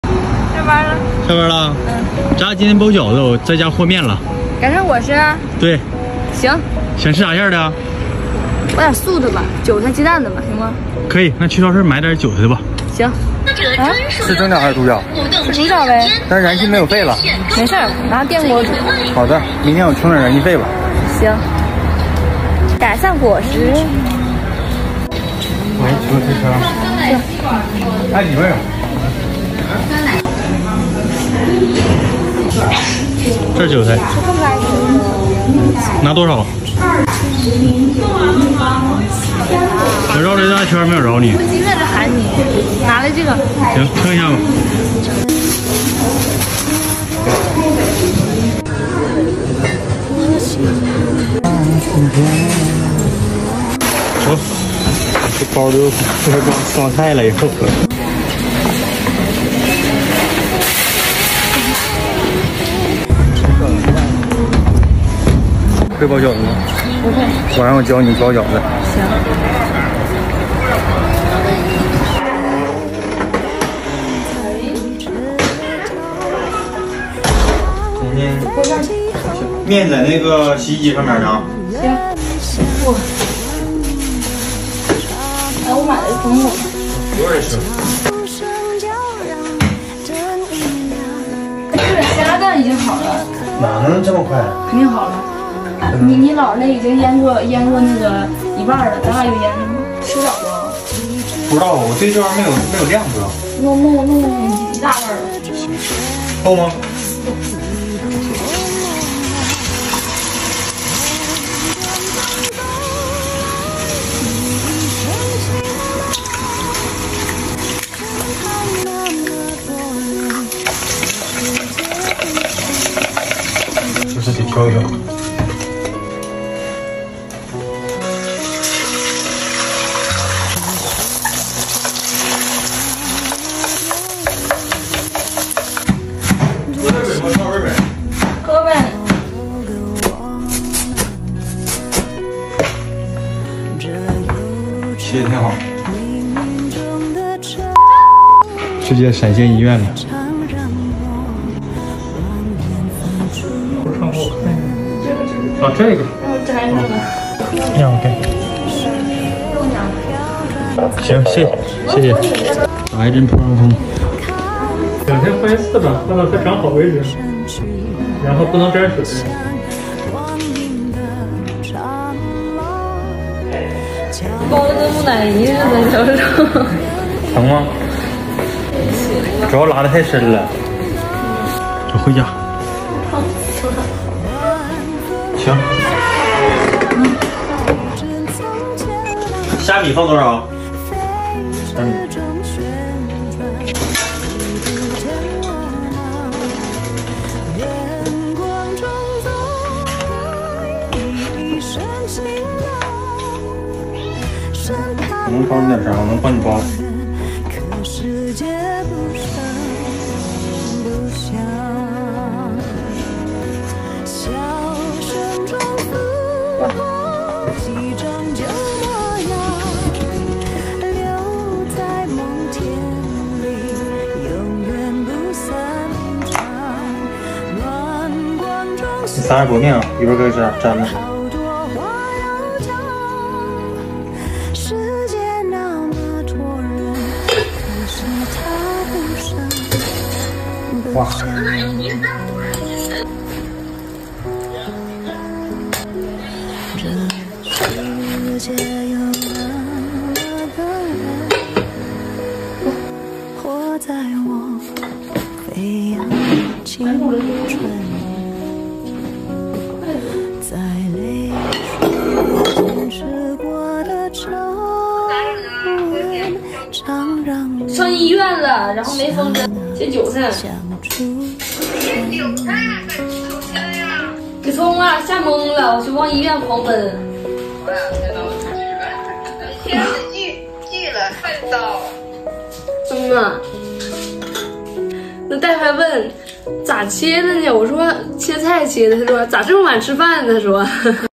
在北京下班后的生活，两个人一起下班，一起去超市买菜，一起回家包饺子，虽然中途出了点意外，但是也不影响两个人一起努力生活#记录真实生活
90后情侣的北漂vlog ！
希望我们能从无到有
四季交替 我们还是我们

The Northern Drifting vlog of 90s Couples!
I hope we can grow from scratch
Four seasons alternate, we are still us